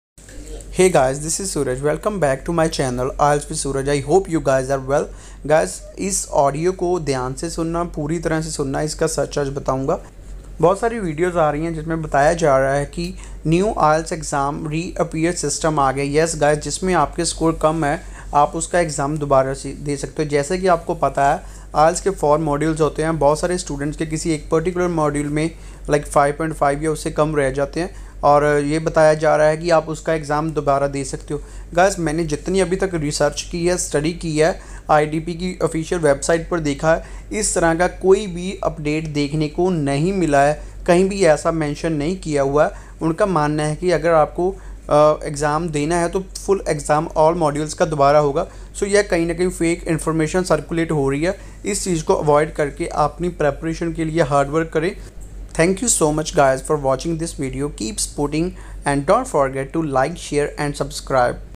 Shocking news:- Ielts students can reappear for one module now? Real or fake? Must watch|Suraj IELTS
THIS VIDEO IS ABOUT CLARIFICATION OF SINGLE MODULE REAPPEAR DOUBT